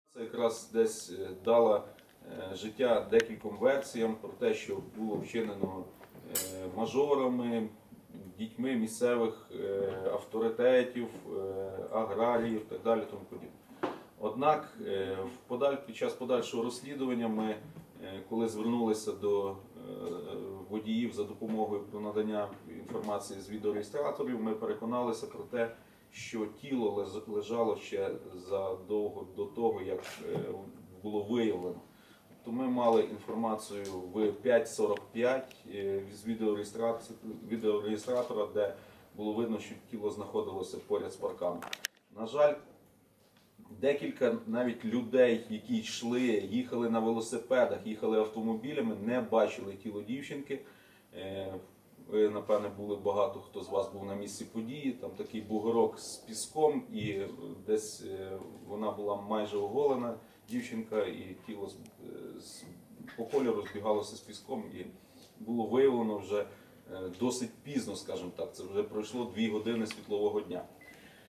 Тіло дівчинки лежало біля траси -там її і вбили
Підозрюваного у вбивстві Іринки Мукоїди вже затримали і обрали міру запобіжного заходу – тримання під вартою, без права внесення застави.